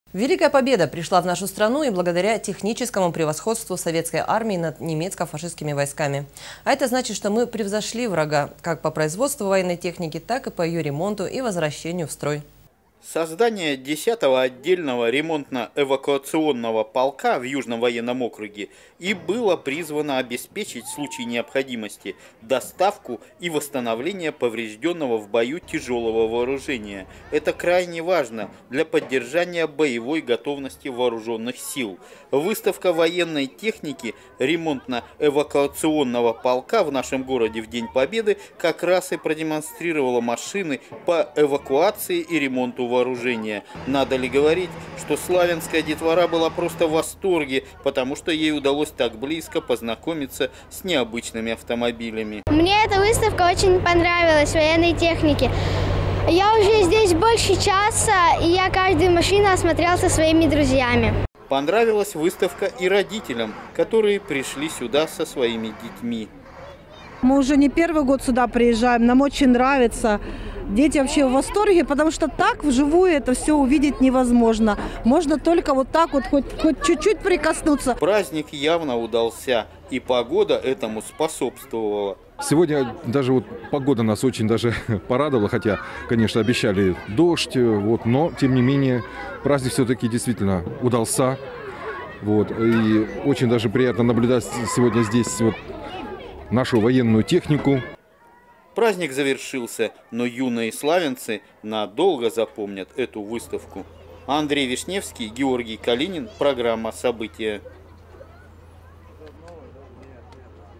Военная техника
Великая победа пришла в нашу страну и благодаря техническому превосходству советской армии над немецко-фашистскими войсками.  А это значит, что мы превзошли врагов,  как по производству военной техники, так и по ее ремонту и возвращению в строй.